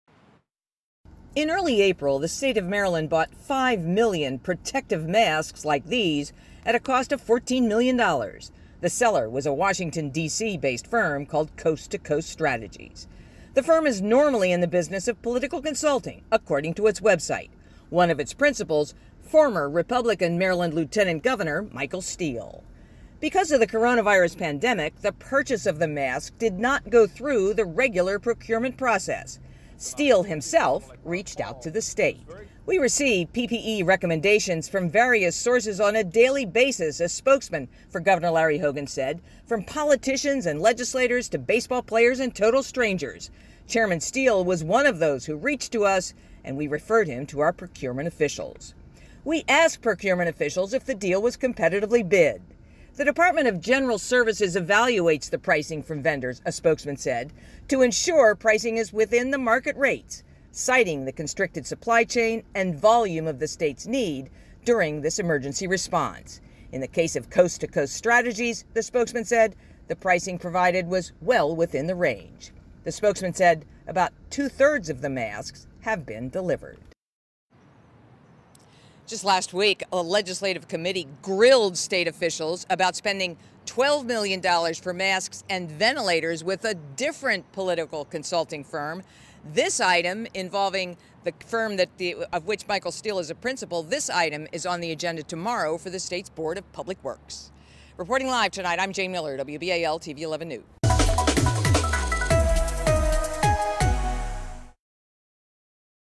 State buys masks from politically connected firm linked to Michael Steele
There may be new questions about Maryland's use of a politically connected firm to buy emergency supplies to fight the coronavirus pandemic, the 11 News I-Team has learned. In early April, the state bought 5 million protective masks at a cost of $14 million from a Washington, D.C.-based firm called Coast to Coast Strategies. It's a firm normally in the business of political consulting, according to its website, and one of its principals is former Republican Maryland Lt. Gov. Michael Steele.